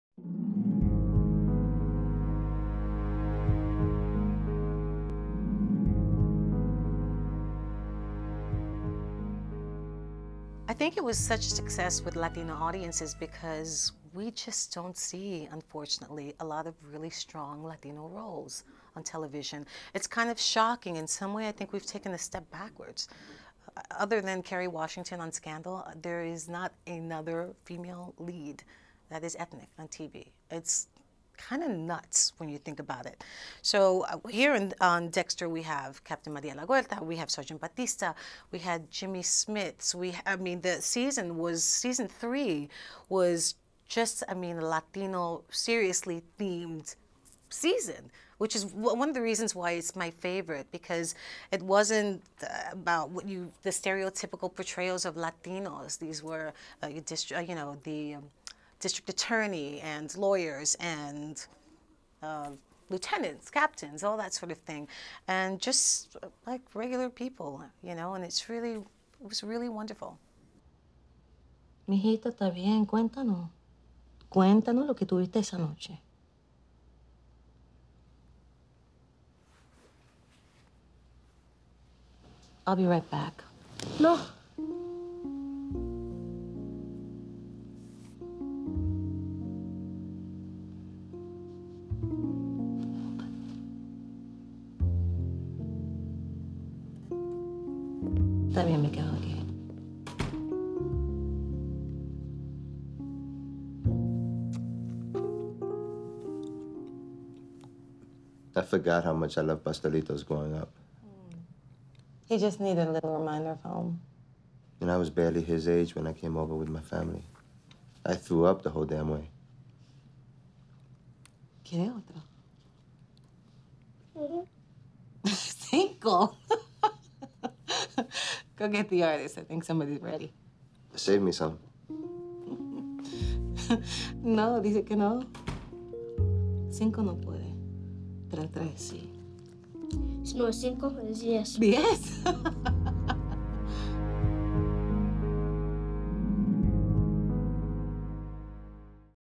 Lauren Velez talks Dexter's success amongst Latinos|Dexter on NUVOtv
Catch Dexter every Monday 9|8c on NUVOtv!